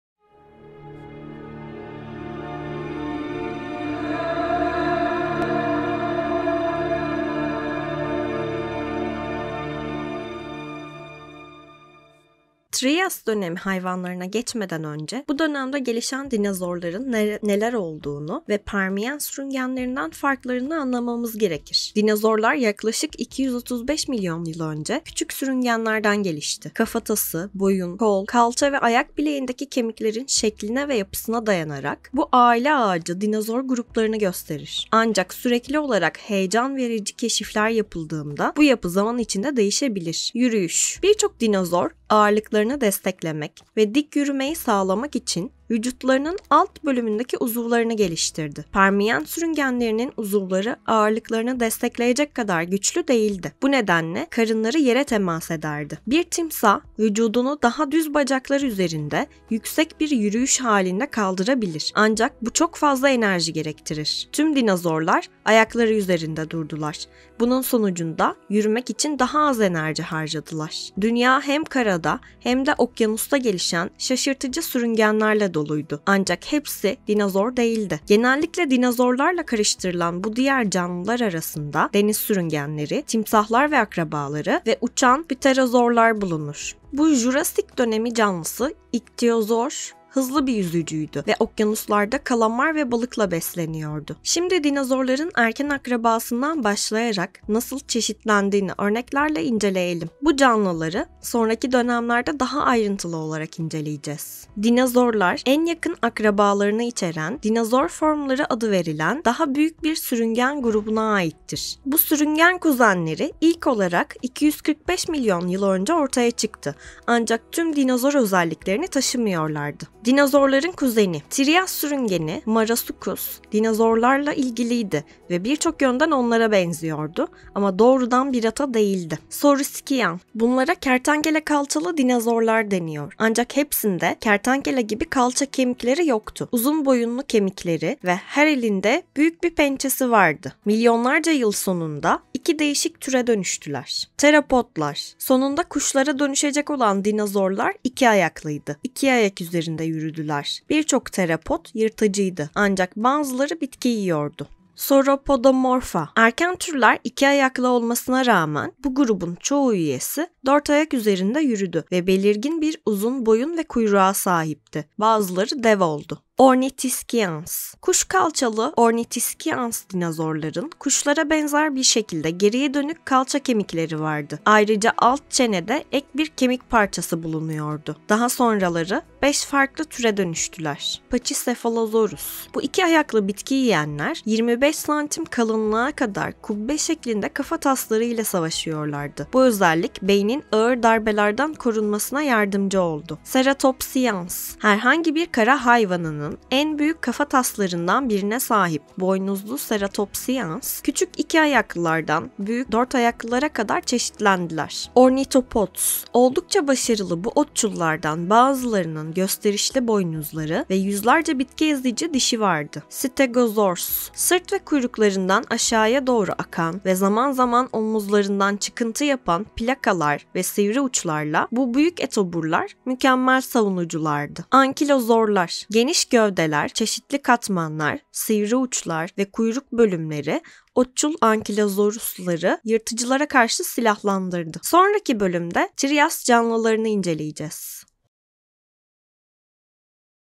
Dinozor nedir?
Dinozorlar yaklaşık 235 milyon yıl önce küçük sürüngenlerden gelişti. Kafatası, boyun,  kol, kalça ve ayak bileğindeki kemiklerin şekline ve yapısına dayanarak, bu aile ağacı  dinozor gruplarını gösterir. Ancak, sürekli olarak heyecan verici keşifler yapıldığında,  bu yapı zaman içinde değişebilir.